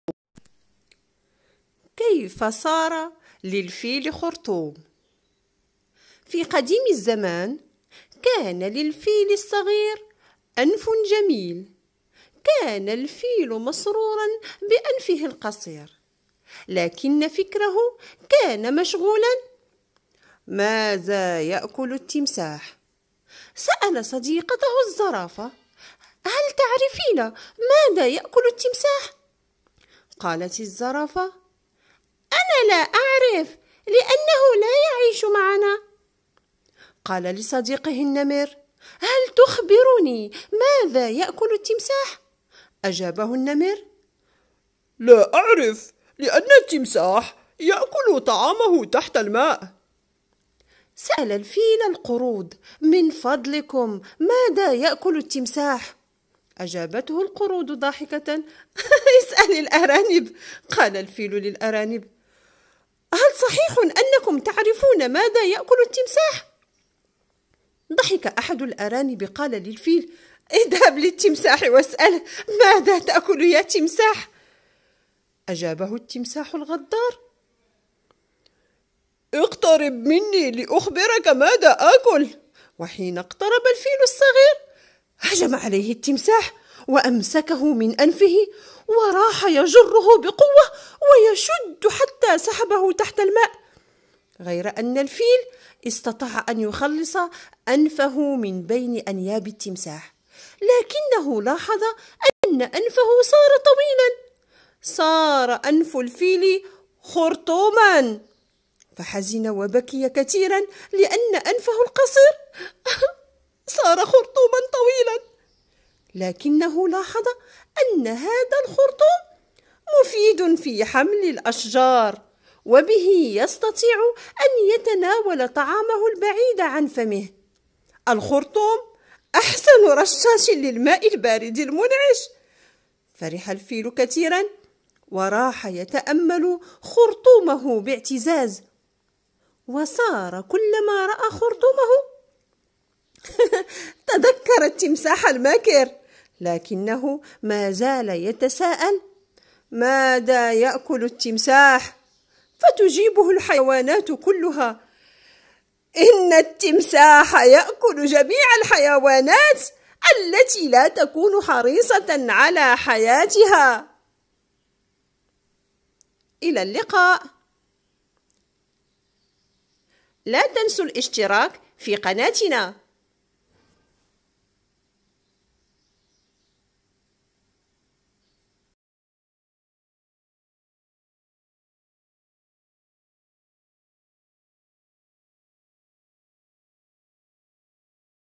كيف صار للفيل خرطوم🤔🤔 للسنة الثانية ابتدائي
النص السماعي :كيف صار للفيل خرطوم. للسنة الثانية ابتدائي من كتاب مرشدي في اللغة